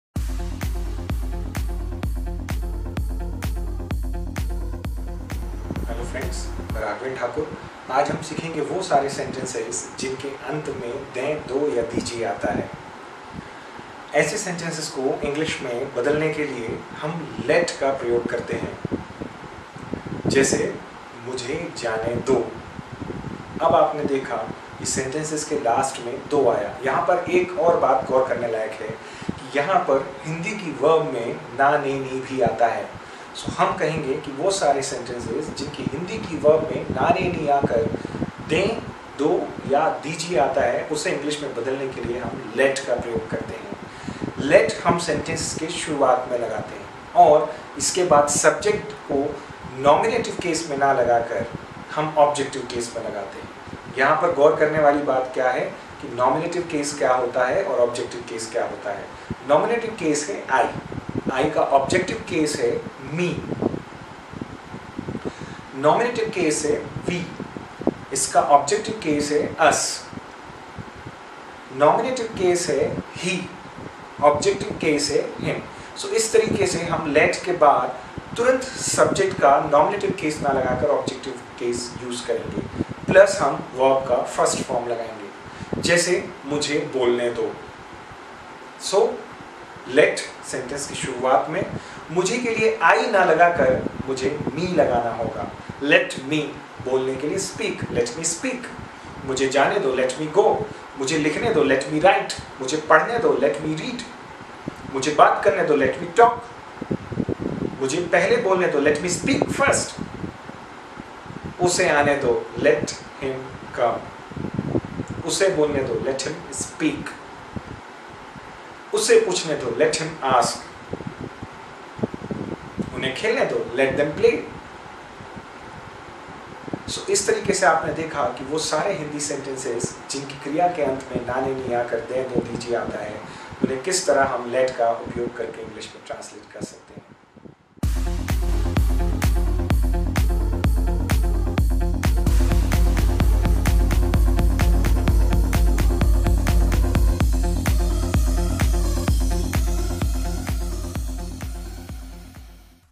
Spoken English में "Let" का प्रयोग (" बोलने दो/जाने दो /लिखने दो " जैसे वाक्यों को बनाना )
Learn how to use "Let" in spoken with Raghvendra Sir in a very easy method without typical grammar tactics.

नीचे कुछ और की  लिंक  दी जा रही है जिसकी सहायता से आप English के और  भी videos देखकर Spoken English आसानी से सीख सकते हैं –

24.  Use of "Not to speak of" in Spoken English (" चाय तो चाय/चाय कहना ही क्या "जैसे वाक्यों को बनाना)